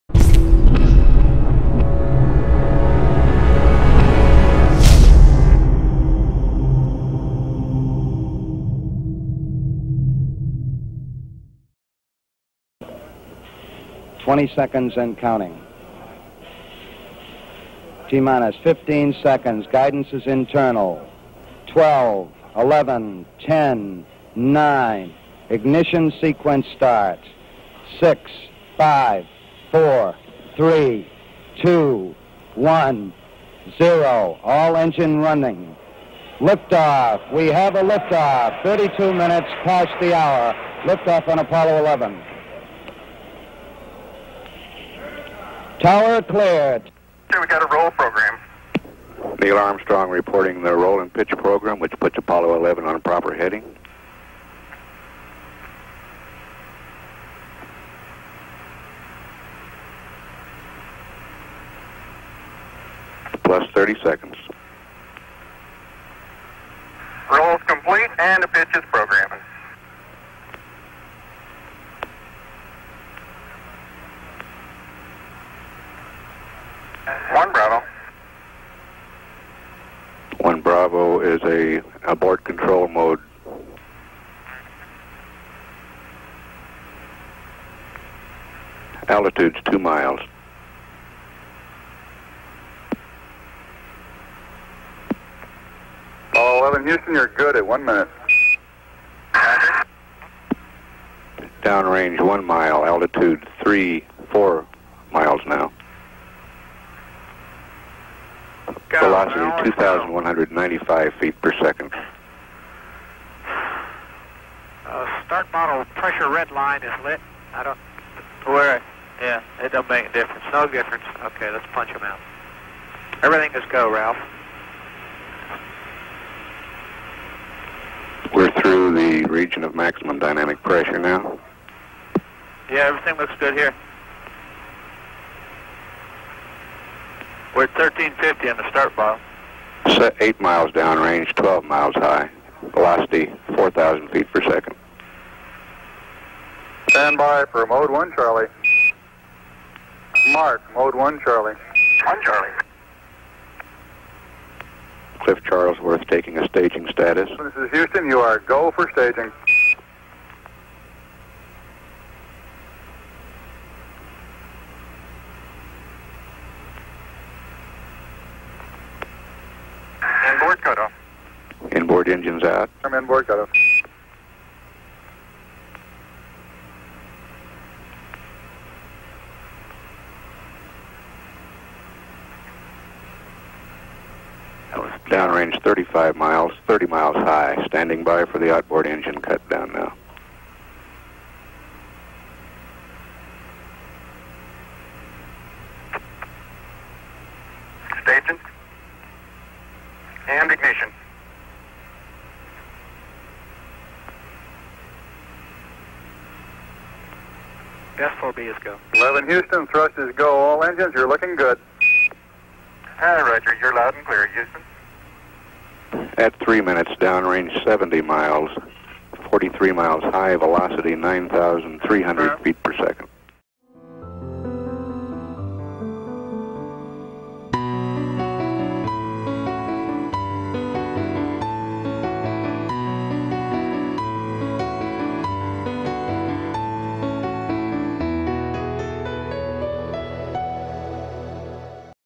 Decolagem da Apollo 11
Vídeo original da decolagem da missão Apollo 11, em 16 de julho de 1969, legendado.

Tripulada pelos astronautas Neil Armstrong, Edwin Buzz Aldrin e Michael Collins, a missão cumpriu o objetivo final do Presidente John F. Kennedy, que, num discurso aos norte-americanos em 1962, estabeleceu o prazo do fim da década para que o programa espacial dos Estados Unidos realizasse este feito.

Créditos:
- Edição: Fred Cox Junior
- Tradução: aetano Júlio Neto
- Legendas: Lucas Berti